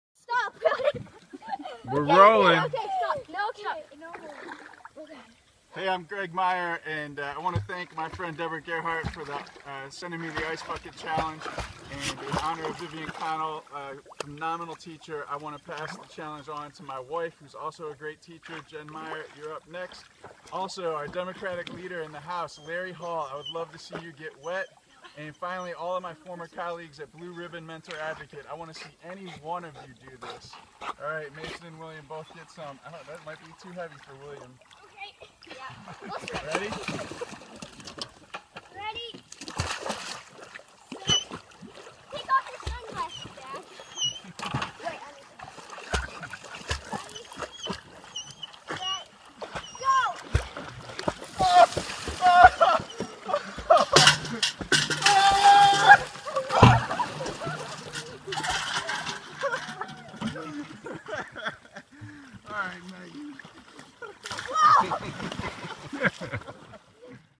Graig Meyer - ALS Ice Bucket Challenge
Accepting the ALS challenge from Deborah Gerhardt and Vivian Connell. I pass it on to Jennifer Meyer, Larry Hall, and the staff of Blue Ribbon Mentor-Advocate.